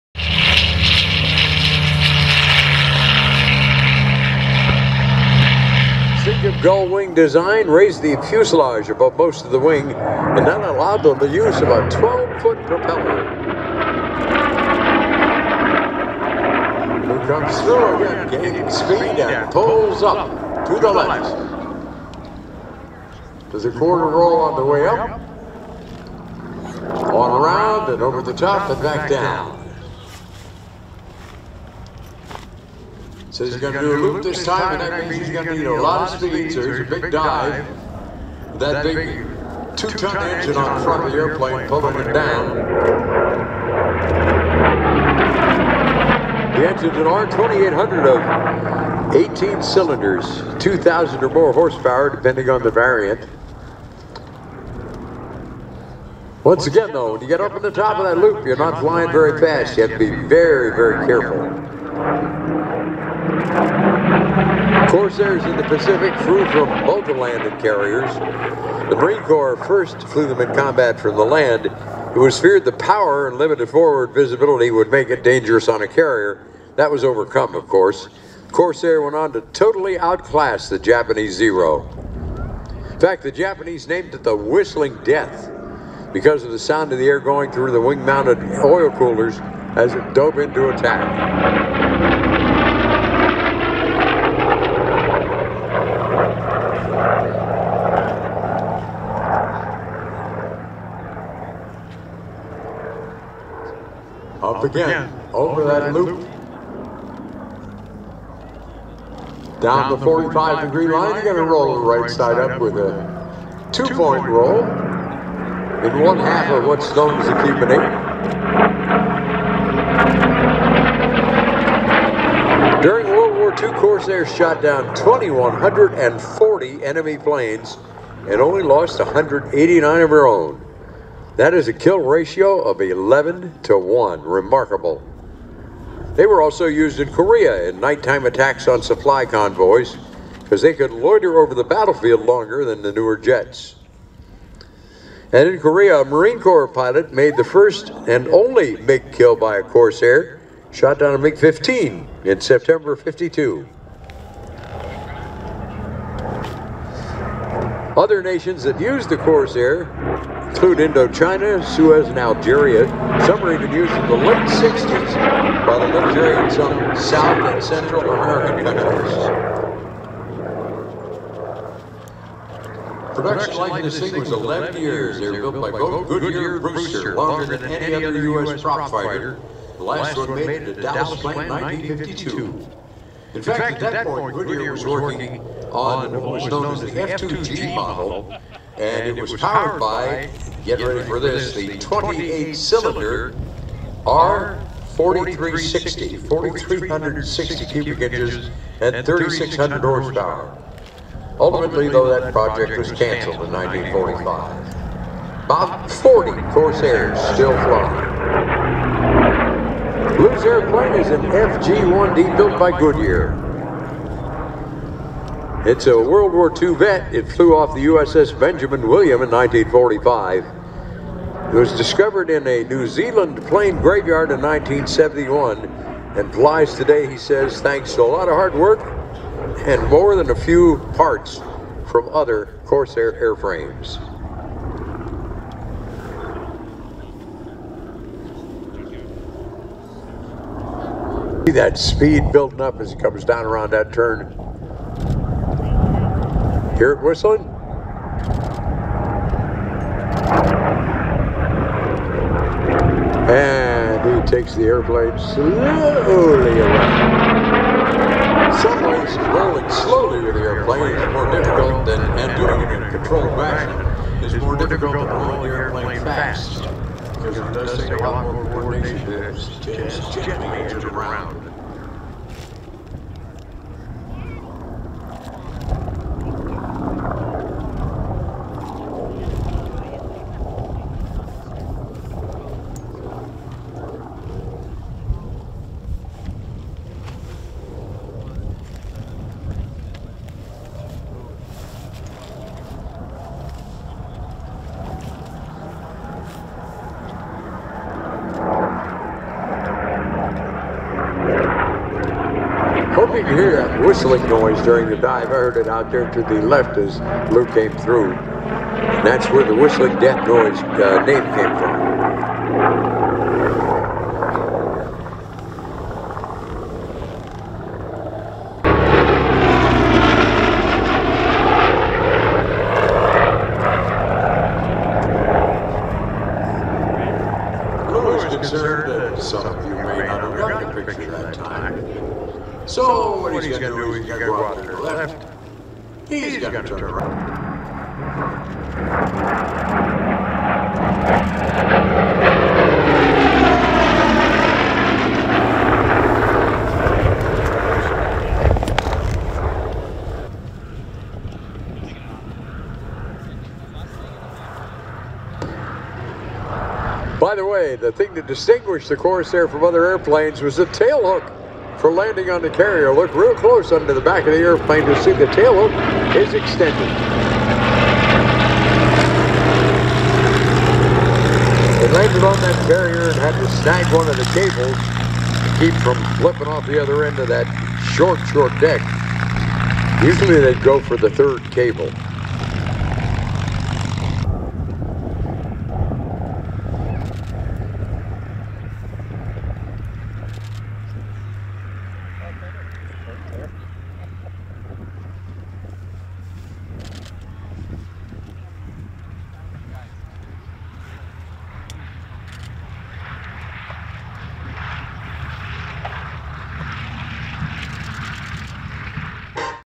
Geneseo Air Show 2022 FG-1D Corsair Demo
Lou Horschel flies his Goodyear FG-1D Corsair at the 2022 Geneseo Air Show at the National Warplane Museum in Geneseo, NY. Saturday June 4, 2022.
The Goodyear FG-1D is a license-built version of the Vought F4U-1D. Brewster Aircraft also built Corsairs during WWII as the F3A.